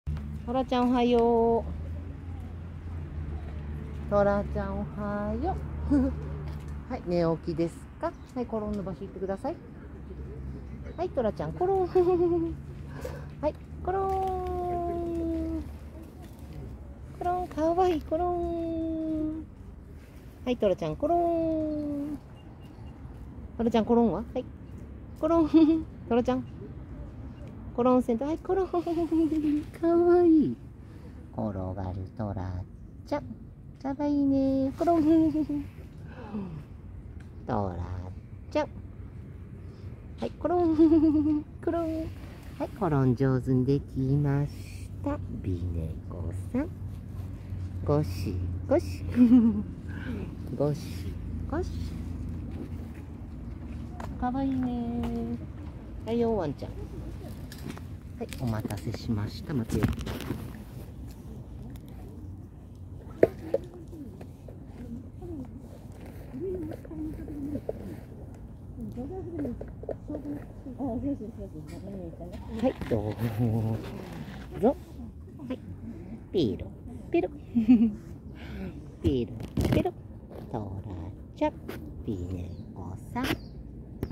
呼ぶと上から降りてきた美猫のトラちゃん❗お決まりの場所まで行きコロンコロン⤴️伏せてご飯待ってるトラちゃん可愛いでしょ❤️
呼ぶと上から降りてきた美猫のトラちゃん❗
お決まりの場所まで行きコロンコロン⤴️
伏せてご飯待ってるトラちゃん可愛いでしょ❤️

When I called Tora-chan, the beautiful cat, he came down from above❗
He went to his usual spot and started crowing⤴️
Isn't Tora-chan cute as he lays down and waits for his food❤️